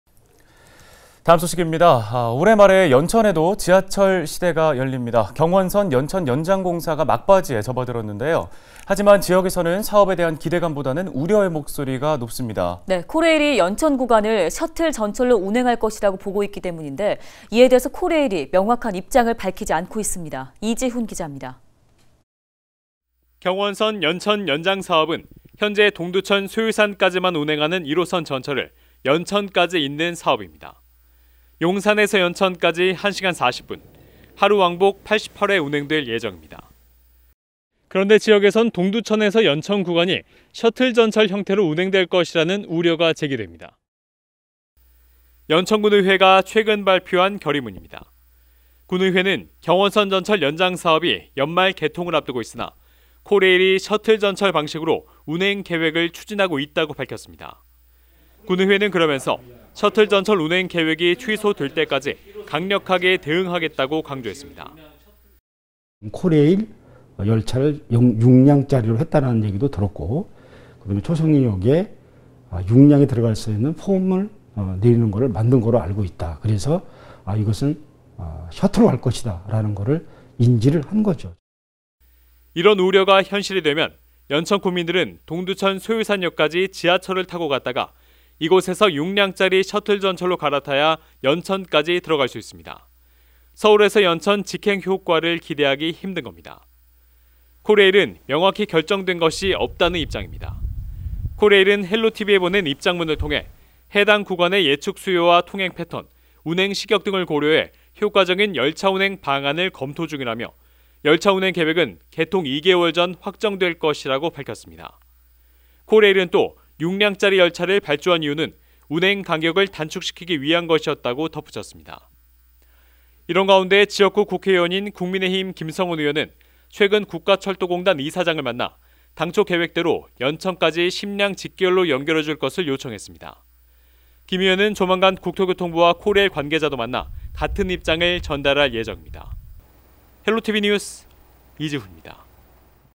경원선 연장선 올해 말 개통···연천군 "셔틀 전철 결사반대"
경원선 연천 연장선이 올해 말 개통 예정인 가운데 지역에서 셔틀 전철 운행을 반대하는 목소리가 높습니다. 반면 코레일은 아직까지 결정된 것이 없다는 입장입니다.